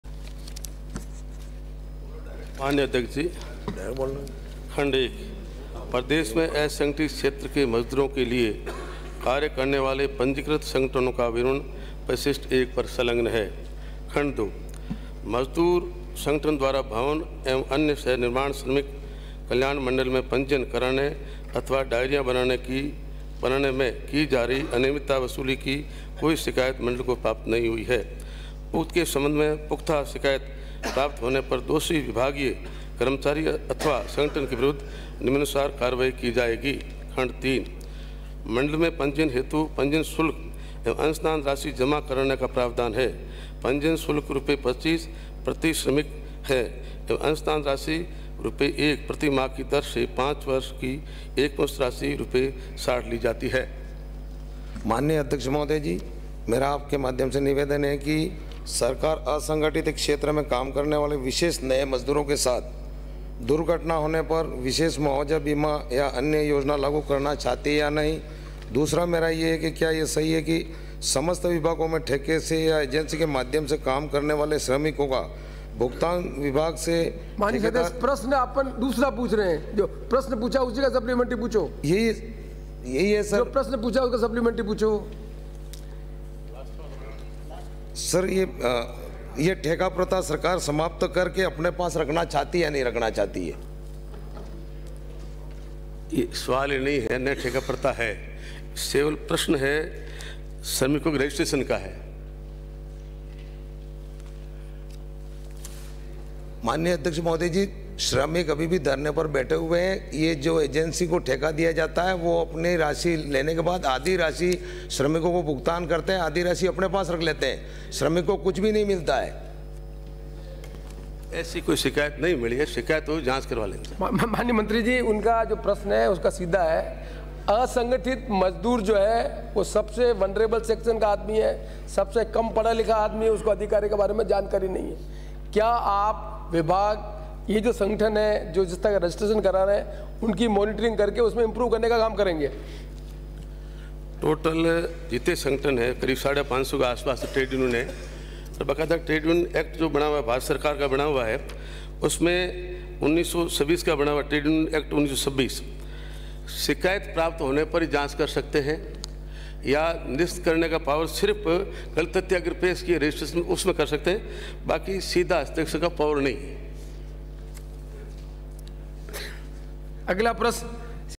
प्रदेश में मजदूर संगठनों द्वारा मजदूर डायरी के शुल्‍क की अवैध वसूली | कैलाशचन्‍द्र मीणा
प्रदेश में मजदूर संगठनों द्वारा मजदूर डायरी के शुल्‍क की अवैध वसूली | कैलाशचन्‍द्र मीणा | Kailash Chand Meena